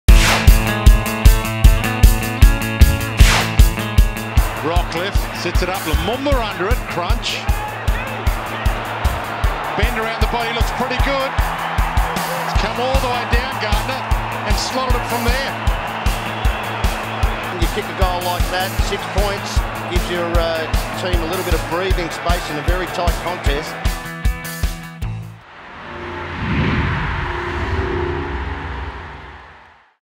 Goals of the year: #7
Number 7: Darcy Gardiner scores to extend Brisbane's lead over Collingwood in round 21.